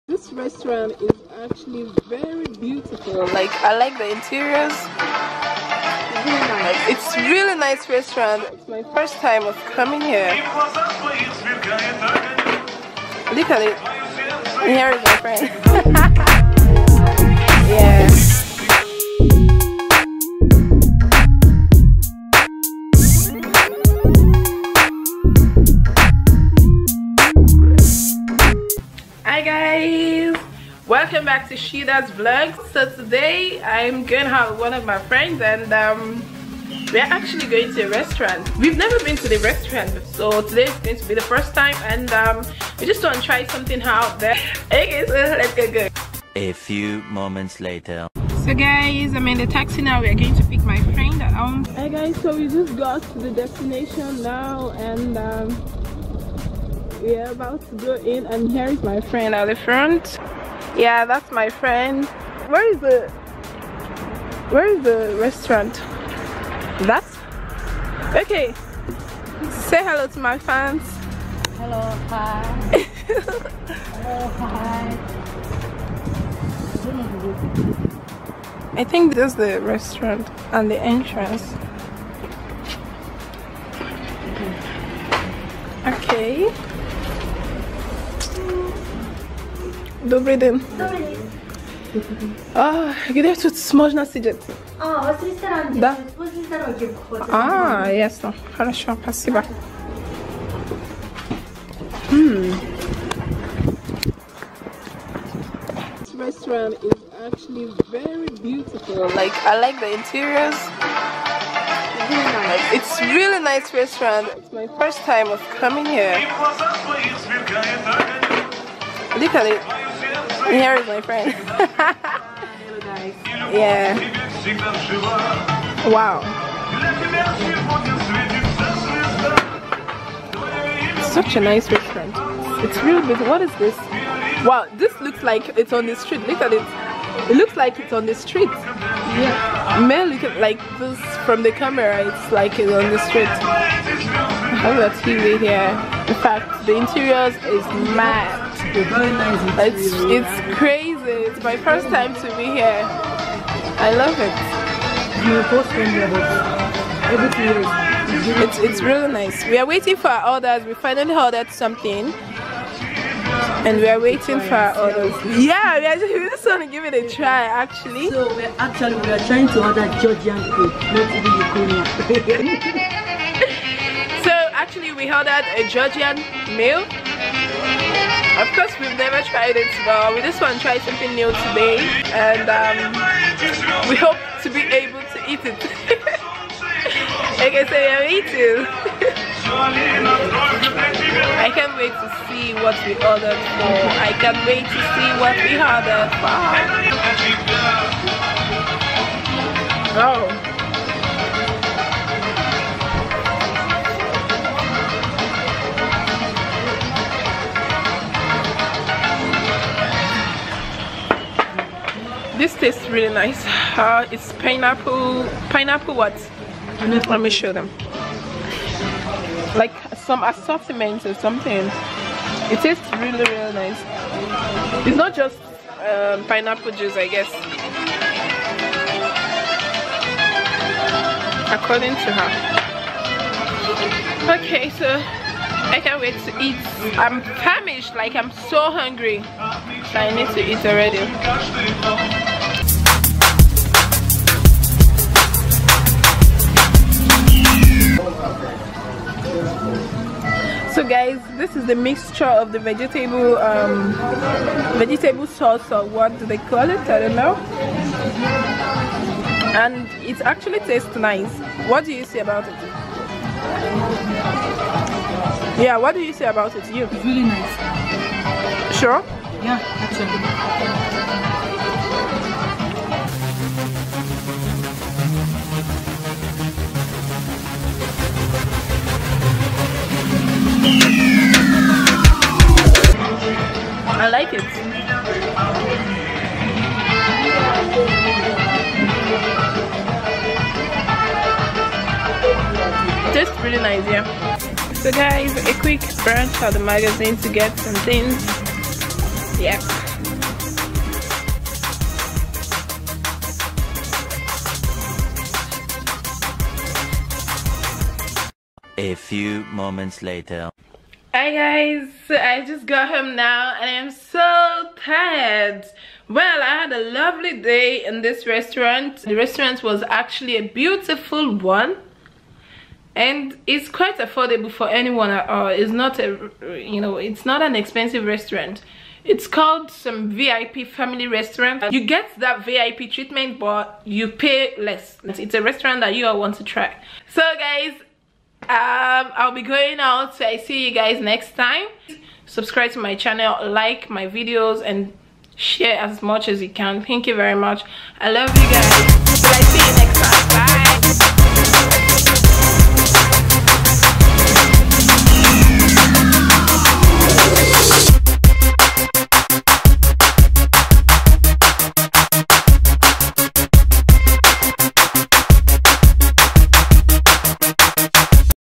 A Day Out With Friend
My first time at this restaurant with my friend  was really amazing... Ask me in the comment section for more information  about it if you wish to visit the restaurant...
Im so sorry for the frame size of the video, its the changes i mistakenly made to my camera. Next video will be a normal frame size.